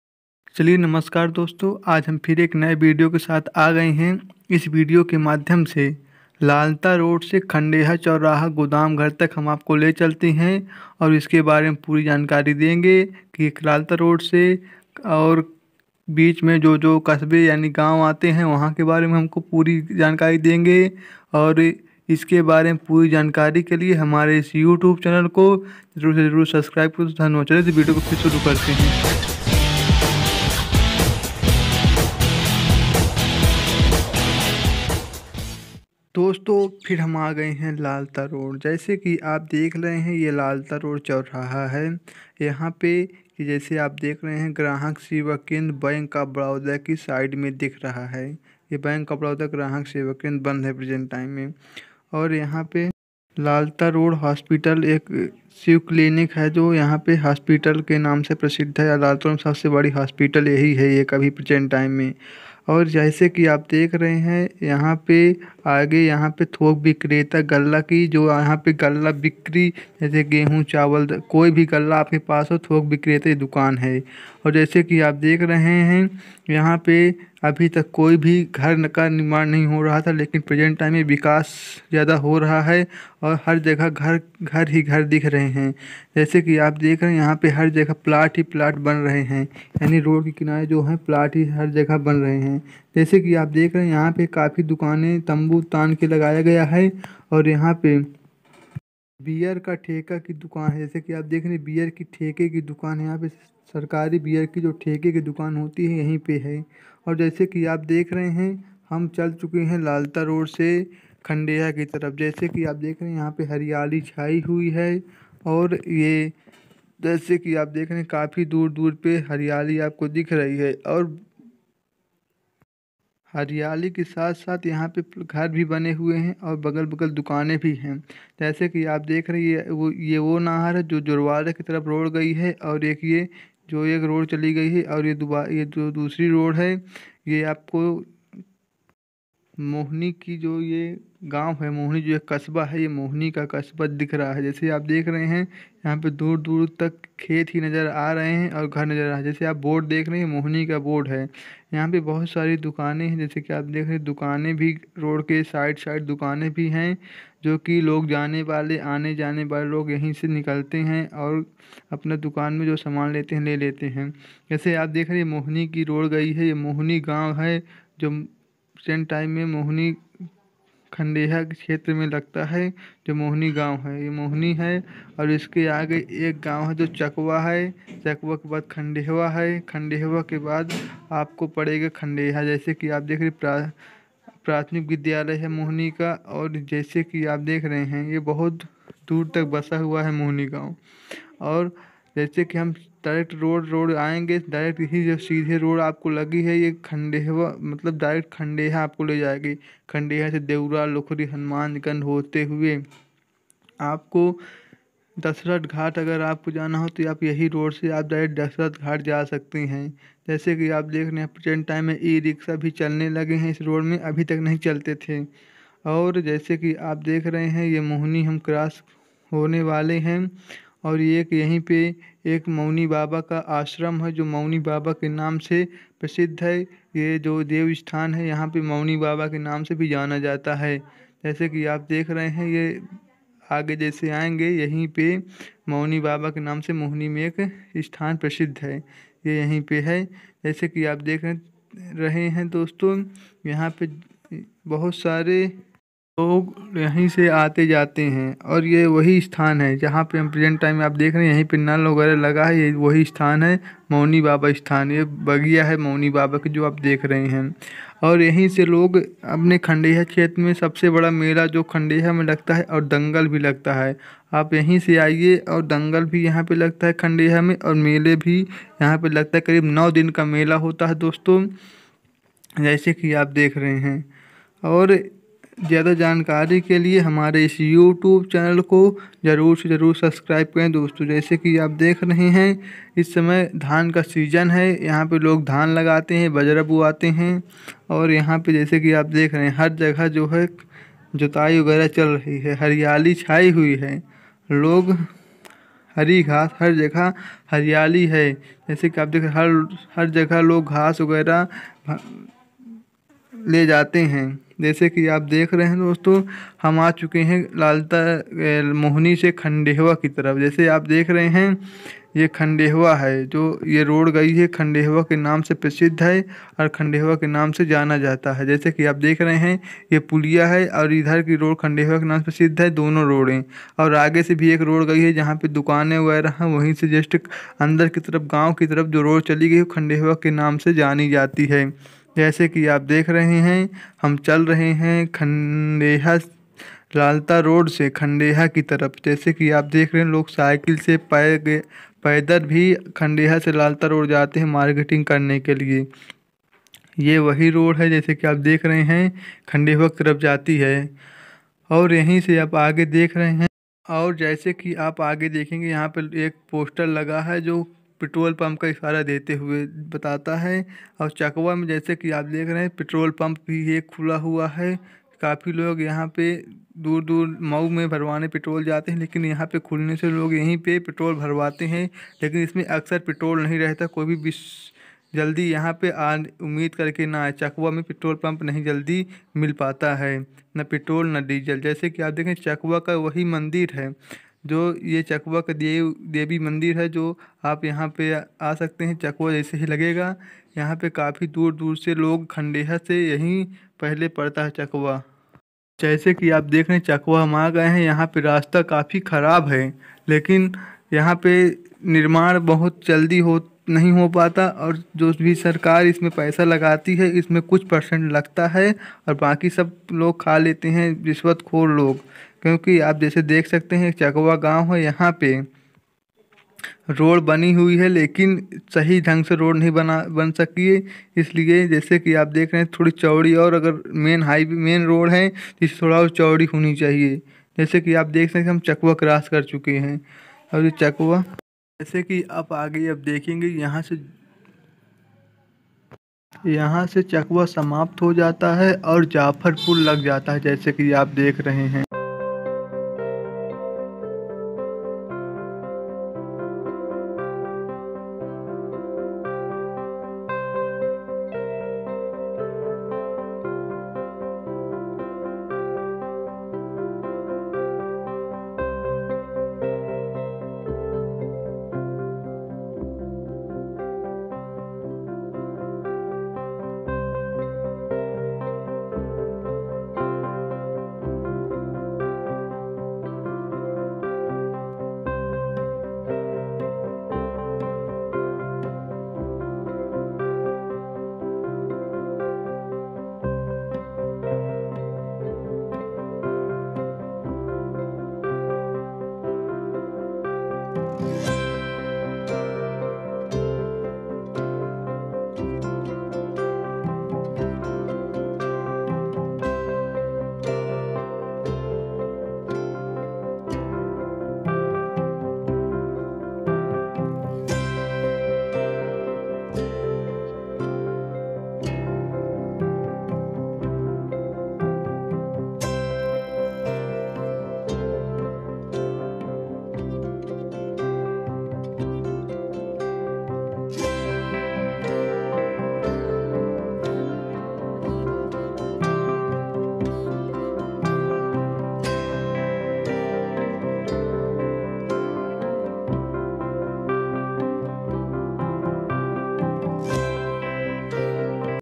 Laltarod to khandeha mau chitrakoot Full jankari information //mohni, chakwa Anoop Gurutek ke sath
Laltarod to khandeha Full information Anoop Gurutek ke sath